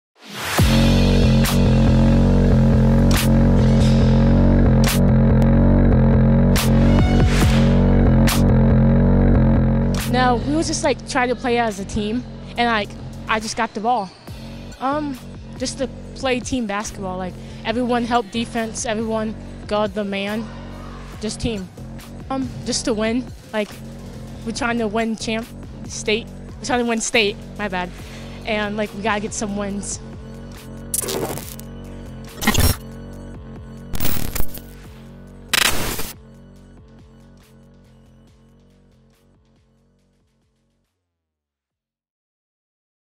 Lake's MVP Serenitee Johnson
Our Whitaker-Myers Wealth Managers, A Registered Investment Advisory Firm MVP from Wooster vs Lake was Lady Blue Streaks Serentiee Johnson who led the team in scoring with 19 points in the win over Wooster 62-49.